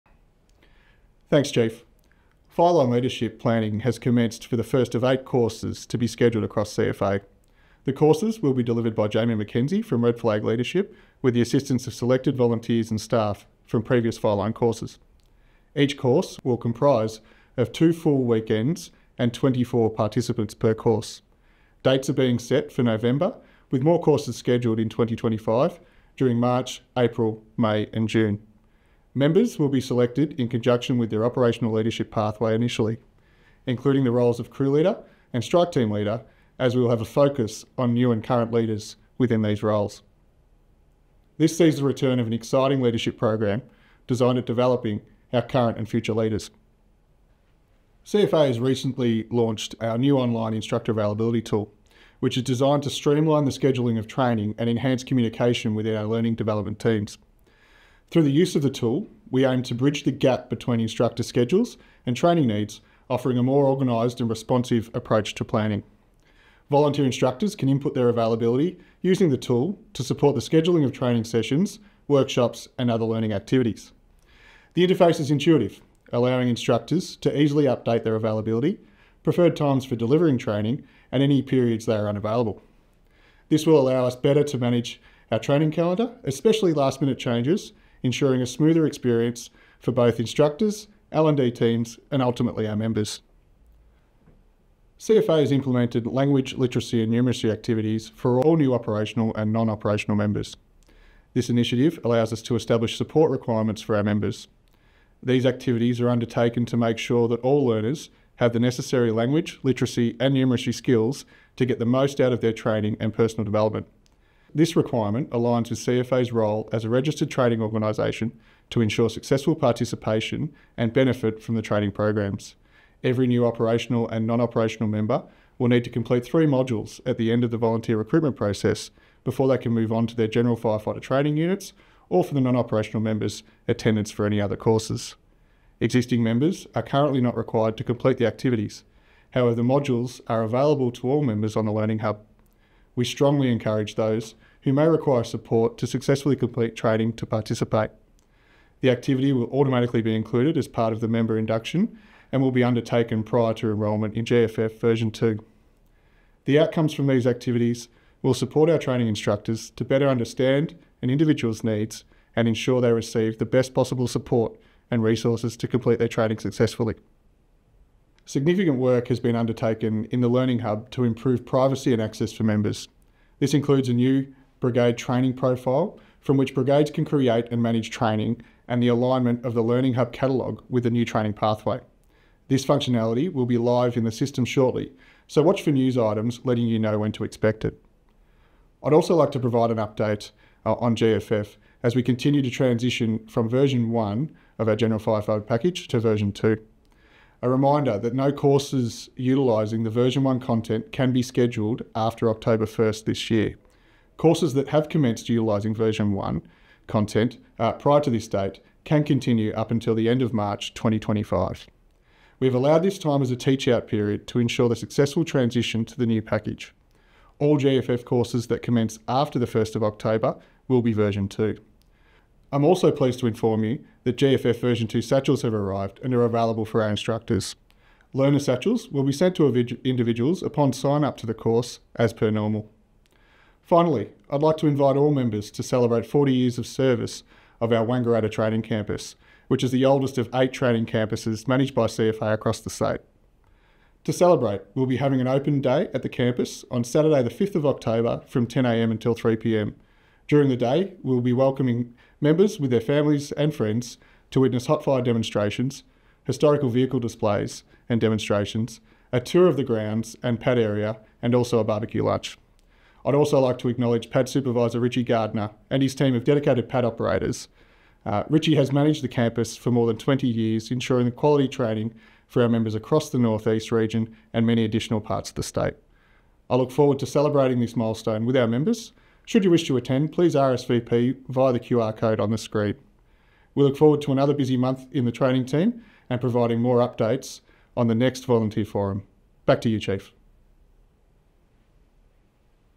Training Update - Sep 24
Acting DCO David Maxwell gives the latest monthly update from the training team.

Owner: ODT
Uploaded by: C&SR